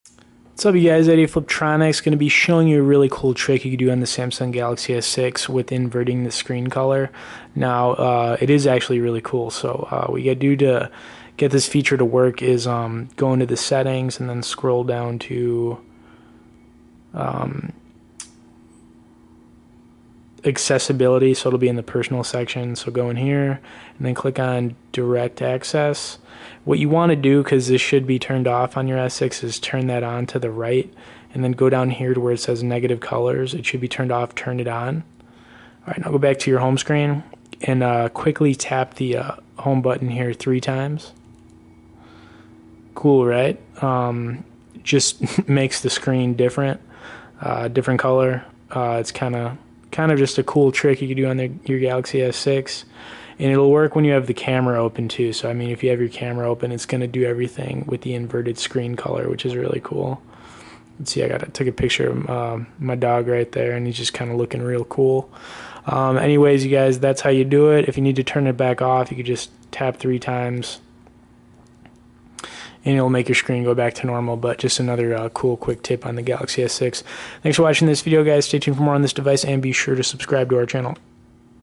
Samsung Galaxy S6: How to Quickly Invert Screen Color With Home Key - Fliptroniks.com
Best Galaxy S6 Case
Best Galaxy S6 Edge Case
Best S6 Wireless Charger
Best S6 Charger

On the samsung galaxy s6 you can invert the screen color that makes it almost look 3d. To do this go into the settings on your galaxy s6 and click on accessibility and then click direct access.  Next make sure the top is turn on to the right and that negative colors is check marked green. Then go back to the home screen and click the home button 3 times quickly and the screen on your samsung galaxy s6 will invert. It will also do this if you open your camera and want to take a picture or shoot a quick video. Its just a cool little trick that you can do on your galaxy s6 so definitely give it a try.